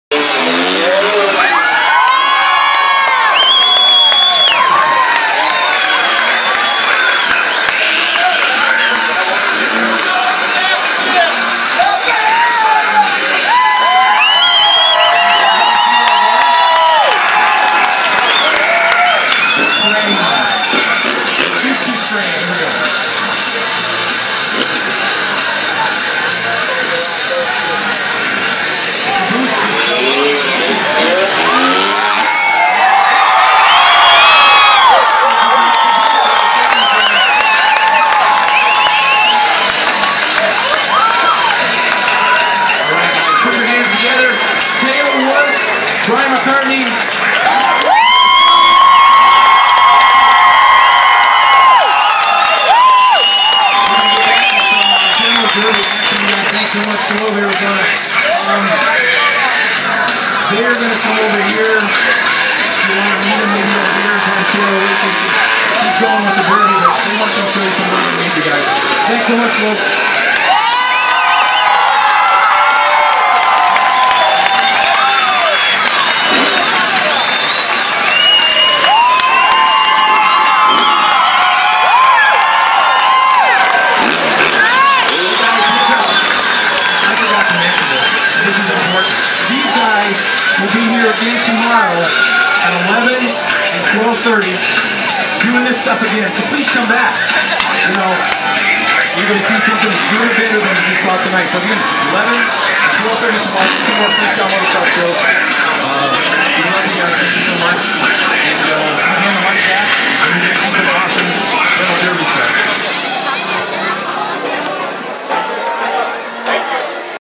Motocross finale
The finale of the motocross event at the Gila county fair